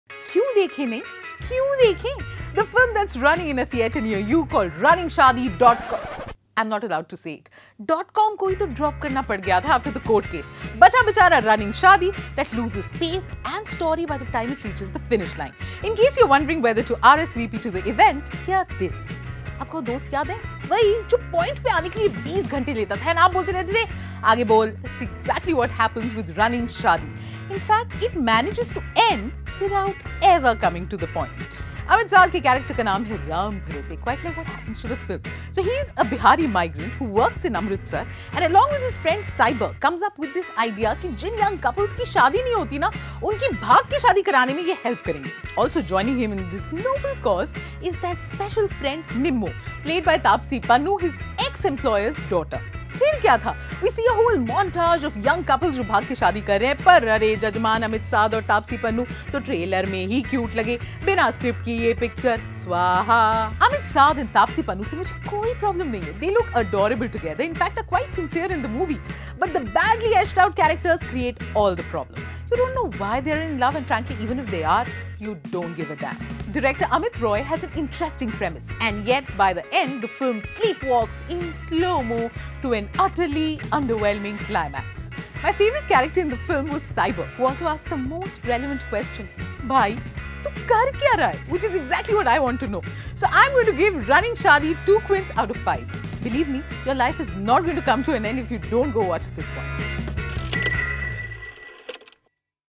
The Quint: Running Shaadi Movie Review
Watch a movie review of latest bollywood film "Running Shaddi" on 'The Quint' youtube channel.